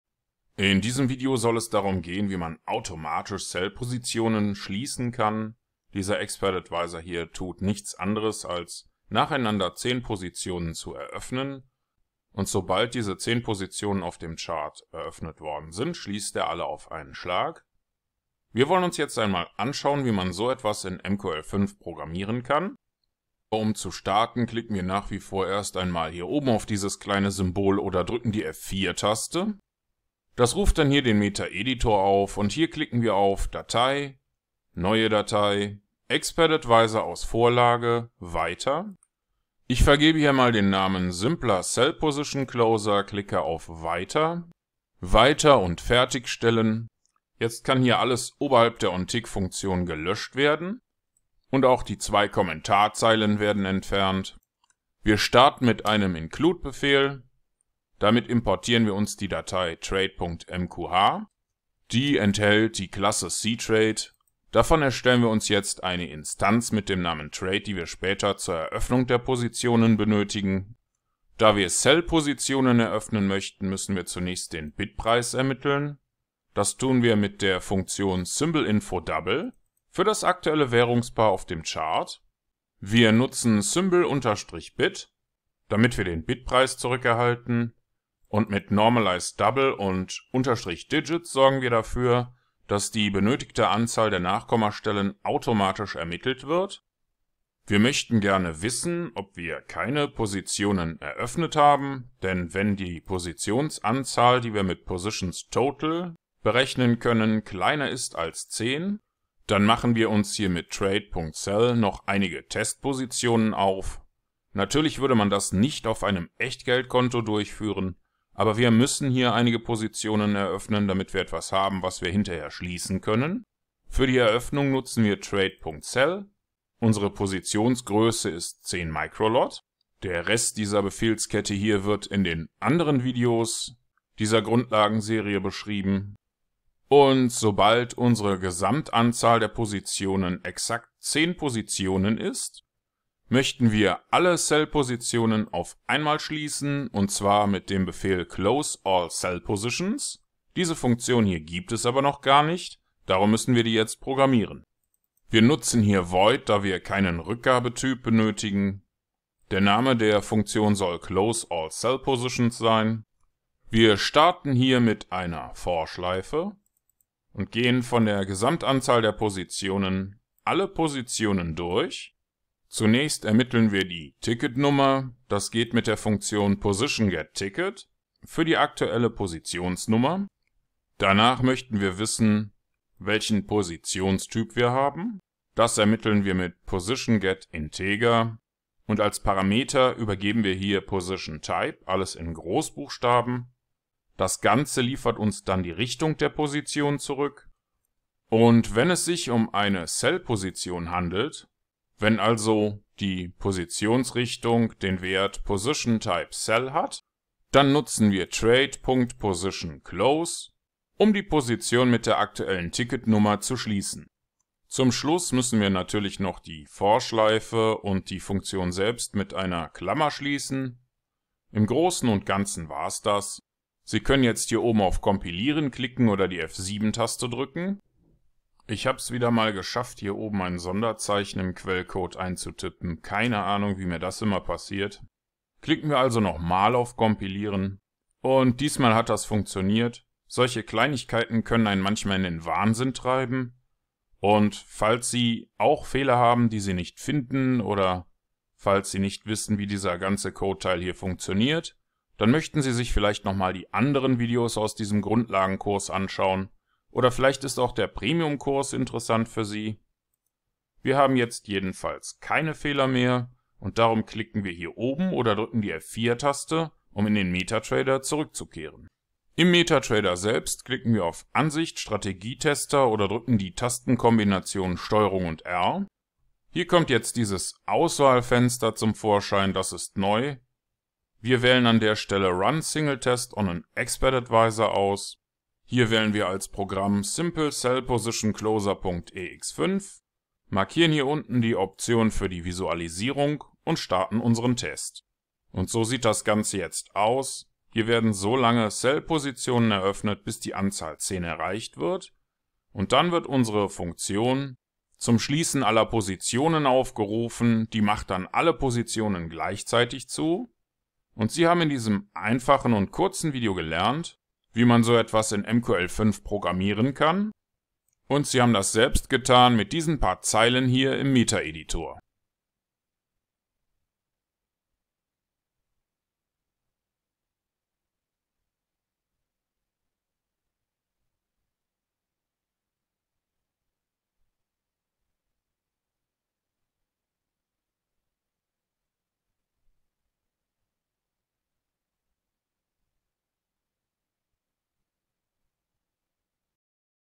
MQL5 TUTORIAL GRUNDLAGEN - 31 SO SCHLIESSEN SIE ALLE SELL POSITIONEN
Mit MQL5 für Metatrader5 können wir einen EA, auch bekannt als Expert Advisor programmieren der auf einem Forex Chart automatisch traden kann. In diesem Beispiel wollen wir herausfinden, wie man mit MQL5 alle offenen Sell Positionen gleichzeitig schliessen kann...

In diesem Video soll es darum gehen, wie man automatisch Sell Positionen schließen kann. Dieser Expert Advisor hier tut nichts anderes, als nacheinander zehn Positionen zu eröffnen. Und sobald diese zehn Positionen auf dem Chart eröffnet worden sind, schließt er alle auf einen Schlag. Wir wollen uns jetzt einmal anschauen, wie man so etwas in MQL5 programmieren kann. Um zu starten klicken wir nach wie vor erst mal hier oben auf dieses kleine Symbol oder drücken die F4 Taste. Das ruft dann hier den Metaeditor auf. Und hier klicken wir auf Datei, Neue Datei, Expert Advisor aus Vorlage, Weiter. Ich vergebe hier einmal den Namen SimplerSellPositionCloser, klicke auf Weiter, Weiter und Fertigstellen. Jetzt kann hier alles oberhalb der OnTick Funktion gelöscht werden. Und auch die zwei Kommentarzeilen werden entfernt. Wir starten mit einem include Befehl. Damit importieren wir uns die Datei Trade.mqh. Die enthält die Klasse CTrade. Davon erstellen wir uns jetzt eine Instanz mit dem Namen trade, die wir später zur Eröffnung der Positionen benötigen. Da wir Sell Positionen eröffnen möchten, müssen wir zunächst den Bid Preis ermitteln. Das tun wir mit der Funktion SymbolInfoDouble für das aktuelle Währungspaar auf dem Chart. Wir nutzen SYMBOL_BID, damit wir den Bid Preis zurückerhalten. Und mit NormalizeDouble und _Digits sorgen wir dafür, dass die benötigte Anzahl der Nachkommastellen automatisch ermittelt wird. Wir möchten gerne wissen, ob wir keine Positionen eröffnet haben. Denn, wenn die Positionsanzahl, die wir mit PositionsTotal berechnen können, kleiner ist als zehn, dann machen wir uns hier mit trade.Sell, noch einige Testpositionen auf. Natürlich würde man das nicht auf einem Echtgeldkonto durchführen. Aber wir müssen hier einige Positionen eröffnen, damit wir etwas haben, was wir hinterher schließen können. Für die Eröffnung nutzen wir trade.Sell. Unsere Positionsgröße ist zehn Mikrolot. Der Rest dieser Befehlskette hier wird in den anderen Videos dieser Grundlagenserie beschrieben. Und sobald unsere Gesamtanzahl der Positionen exakt zehn Positionen ist, möchten wir alle Sell Positionen auf einmal schließen. Und zwar mit dem Befehl CloseAllSellPositions. Diese Funktion hier gibt es aber noch gar nicht. Darum müssen wir die jetzt programmieren. Wir nutzen hier void, da wir keinen Rückgabetyp benötigen. Der Name der Funktion soll CloseAllSellPositions sein. Wir starten hier mit einer Vorschleife und gehen von der Gesamtanzahl der Positionen alle Positionen durch. Zunächst ermitteln wir die Ticket-Nummer. Das geht mir der Funktion PositionGetTicket für die aktuelle Positionsnummer. Danach möchten wir wissen, welchen Positionstyp wir haben. Das ermitteln wir mit PositionGetInteger. Und als Parameter übergeben wir hier POSITION_TYPE, alles in Großbuchstaben. Das Ganze liefert uns dann die Richtung der Position zurück. Und wenn es sich um eine Sell Position handelt, wenn also die Positionsrichtung den Wert POSITION_TYPE_SELL hat, dann nutzen wir trade.PositionClose, um die Position mit der aktuellen Ticketnummer zu schließen. Zum Schluss müssen wir natürlich noch die Vorschleife und die Funktion selbst mit einer Klammer schließen. Im Großen und Ganzen war es das. Sie können jetzt hier oben auf kompilieren klicken oder die F7 Taste drücken. Ich habe es wiedermal geschafft, hier oben ein Sonderzeichen im Quellcode einzutippen. Keine Ahnung, wie mir das immer passiert. Klicken wir also noch mal auf Kompilieren und diesmal hat das funktioniert. Solche Kleinigkeiten können einen manchmal in den Wahnsinn treiben. Und falls Sie auch Fehler haben, die Sie nicht finden, oder falls Sie nicht wissen, wie dieser ganze Code Teil hier funktioniert, dann möchten Sie sich vielleicht nochmal die anderen Videos aus diesem Grundlagenkurs anschauen. Oder vielleicht ist auch der Premiumkurs interessant für Sie. Wir haben jetzt jedenfalls keine Fehler mehr. Und darum klicken wir hier oben oder drücken die F4 Taste, um in dem Metatrader zurückzukehren. Im Metatrader selbst klicken wir auf Ansicht, Strategietester oder drücken die Tastenkombination Ctrl+R. Hier kommt jetzt dieses Auswahlfenster zum Vorschein. Das ist neu. Wir wählen an der Stelle Run single test of an Expert Advisor aus. Hier wählen wir als Programm SimplerSellPositionCloser.ex5. Markieren hier unten die Option für die Visualisierung und starten unseren Test. Und so sieht das Ganze jetzt aus. Hier werden solange Sell Positionen eröffnet, bis die Anzahl zehn erreicht wird. Und dann wird unsere Funktion zum Schließen aller Positionen aufgerufen. Die macht dann alle Positionen gleichzeitig zu.